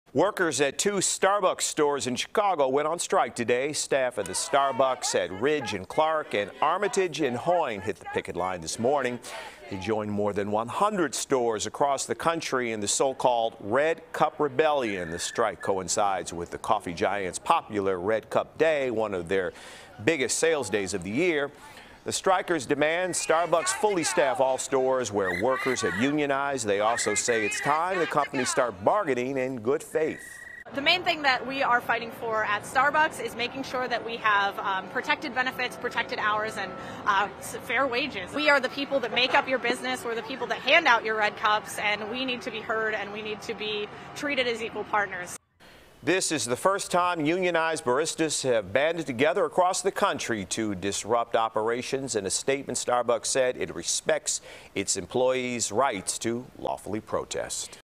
Workers at 2 Chicago Starbucks stores go on strike
The strike coincides with the coffee giant's popular Red Cup Day, one of the their biggest sales days of the year. The strikers demand Starbucks fully staff all the stores where workers have unionized.